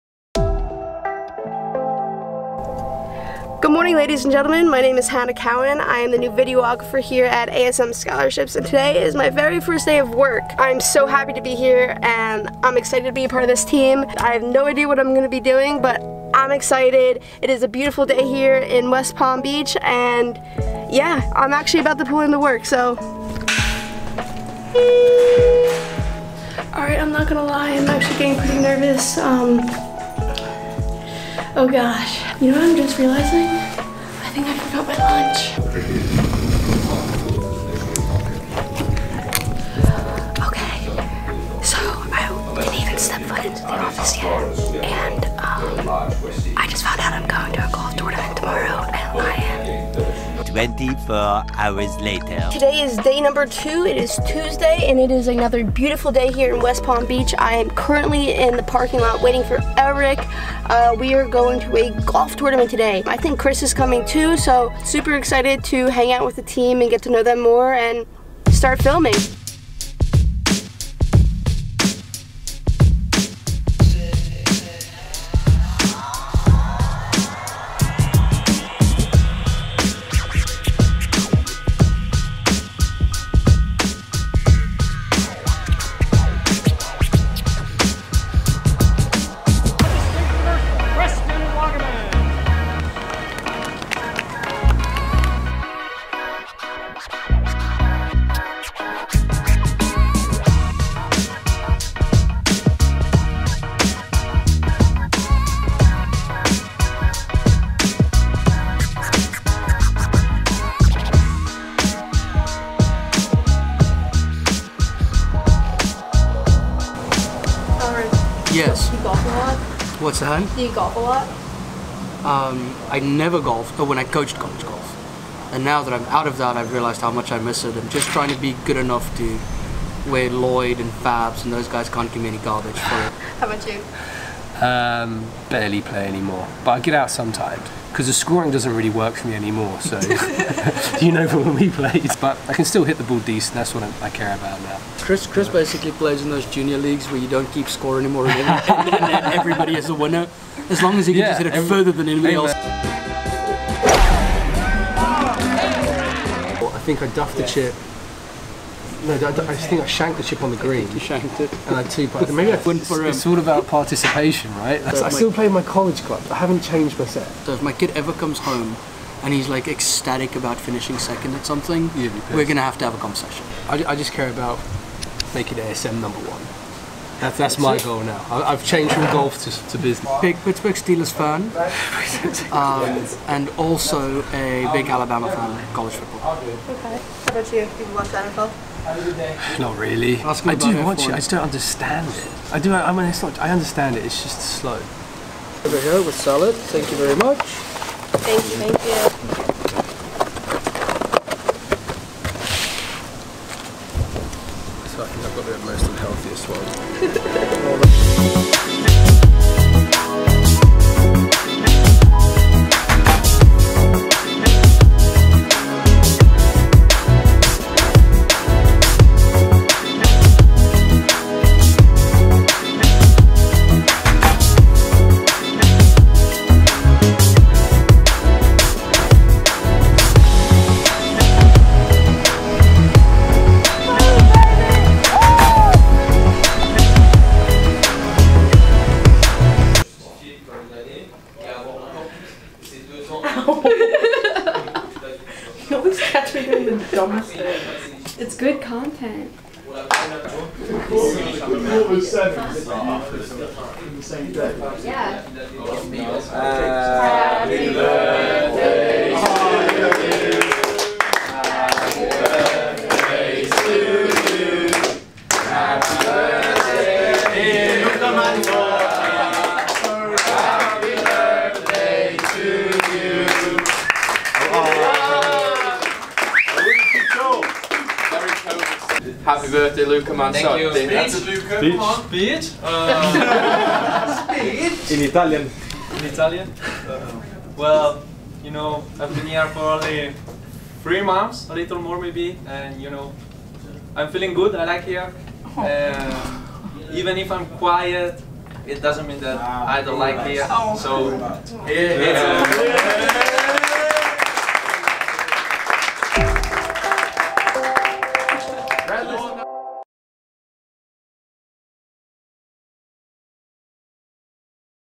NCAA golf championship | ASM Scholarships
This week follow along with our new videographer Hannah Cowan! We went to the 2021 DII men's golf championship at PGA National Resort here in Palm Beach Gardens, Florida. The team met up with some college coaches and we watched Georgia Southwestern play Arkansas Tech for a national title. NCAA men's golf selection show   What a week, also Happy Birthday to our Italian Athlete Consultant, Luca!